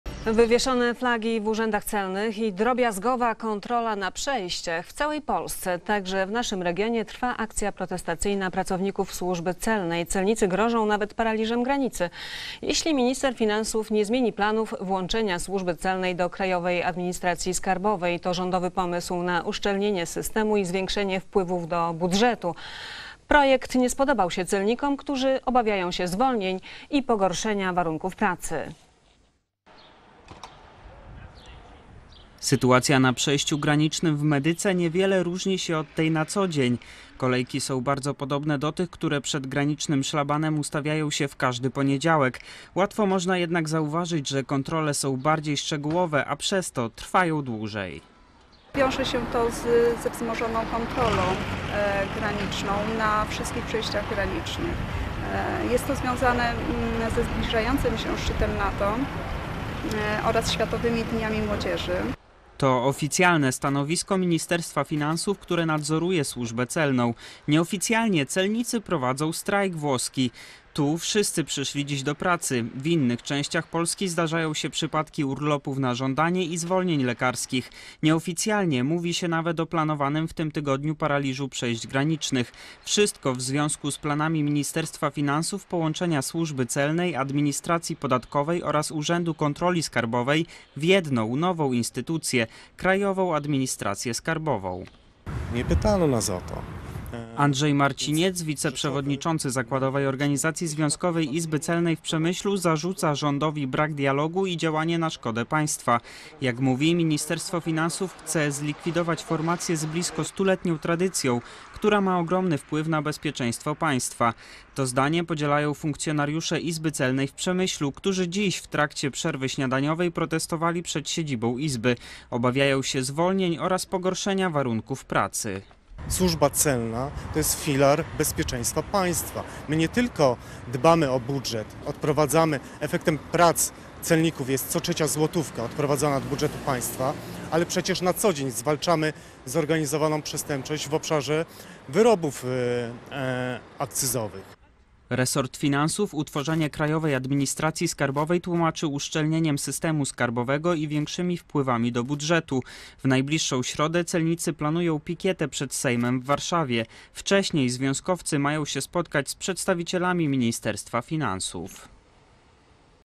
Trwa akcja protestacyjna pracowników służby celnej
Wywieszone flagi w urzędach celnych i drobiazgowa kontrola na przejściach granicznych - w całej Polsce, także w naszym regionie, trwa akcja protestacyjna pracowników służby celnej. Jeśli minister finansów nie zmieni planów włączenia służby celnej do krajowej administracji skarbowej, celnicy grożą nawet paraliżem granicy. To rządowy pomysł na uszczelnienie systemu skarbowego i zwiększenie wpływów do budżetu. Projekt nie spodobał się celnikom, którzy obawiają się zwolnień i pogorszenia warunków pracy.